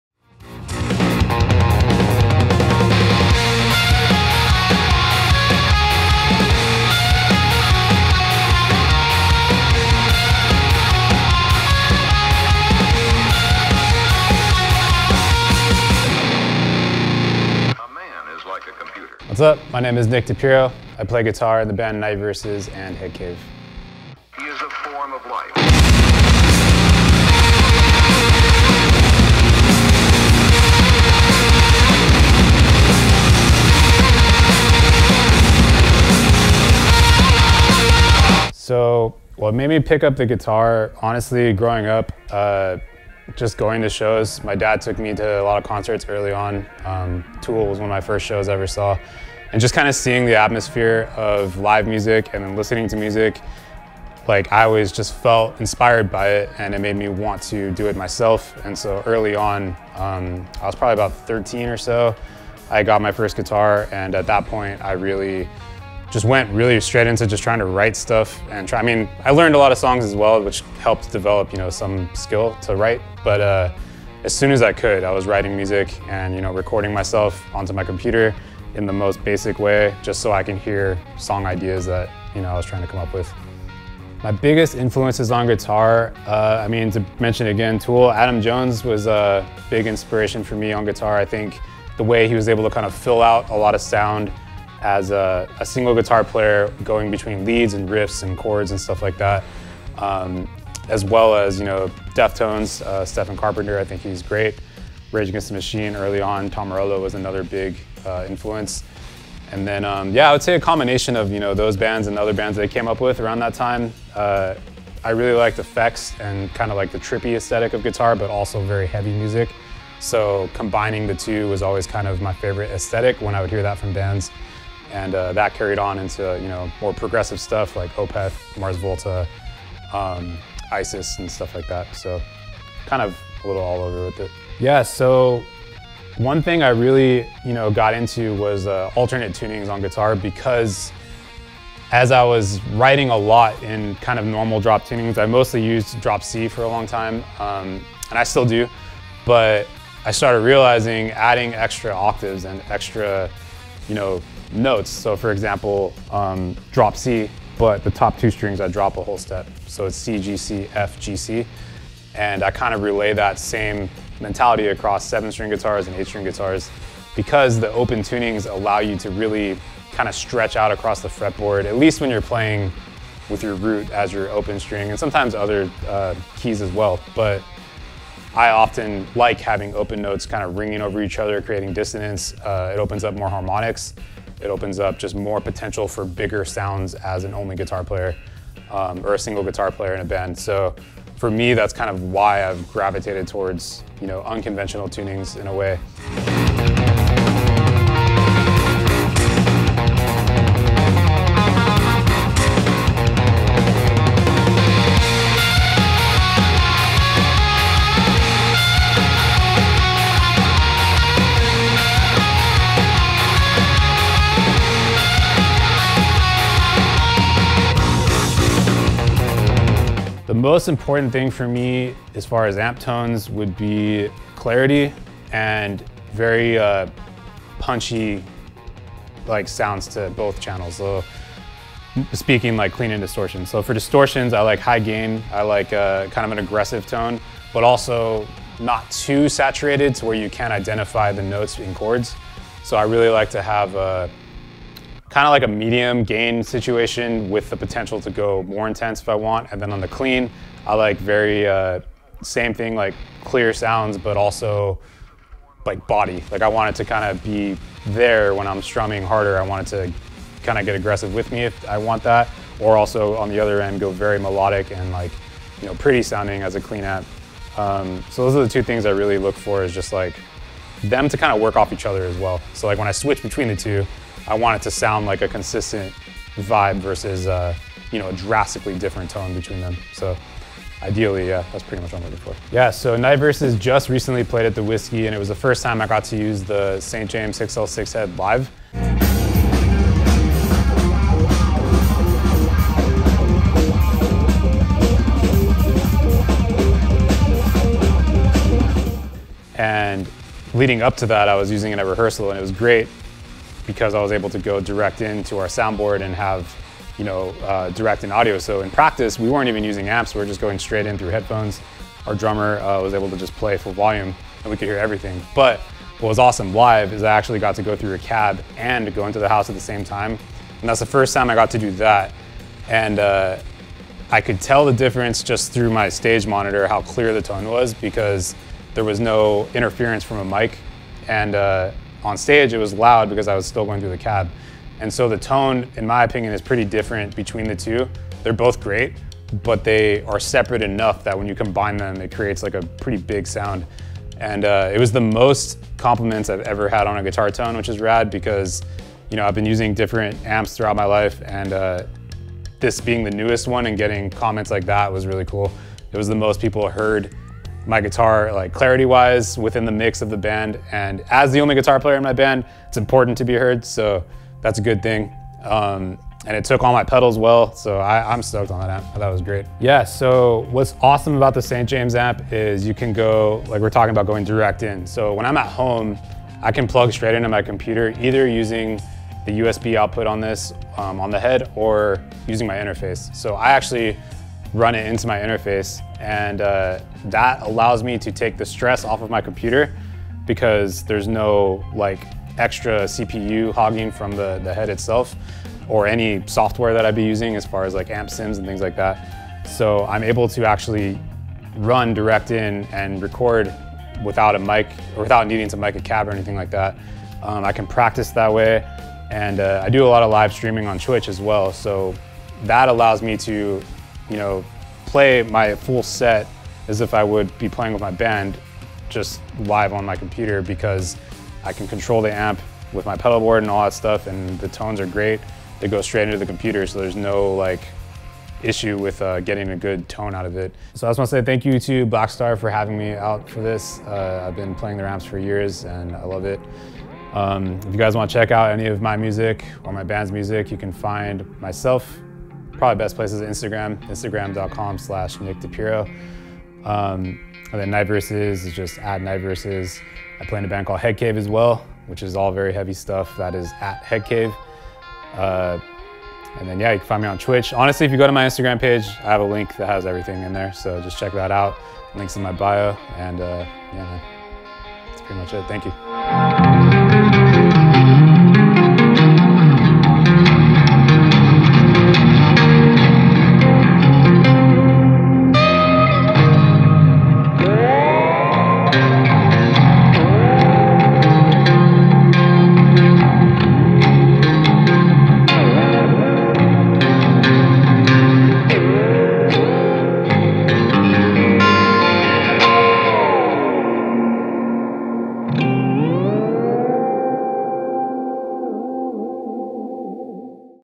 In Conversation With Nick DePirro (Night Verses/Headcave) | St. James | Blackstar
We sat down with Nick DePirro, guitarist in Night Verses & Headcave to chat about some of the St. James' features and tones that have made this his new go-to amplifier.